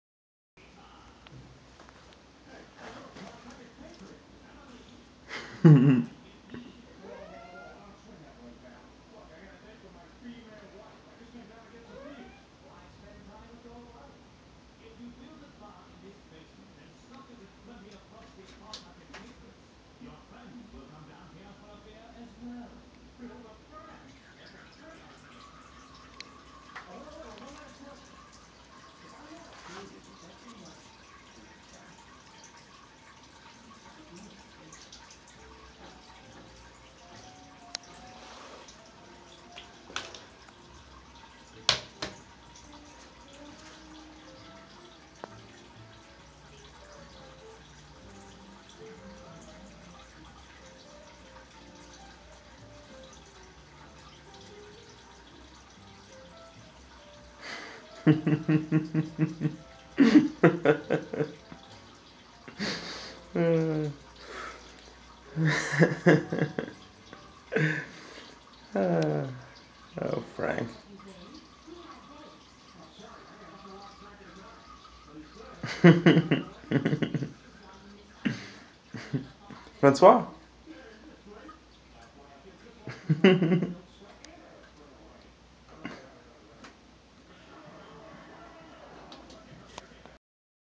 Francois taking a leak on the toilet
My cat can pee in the toilet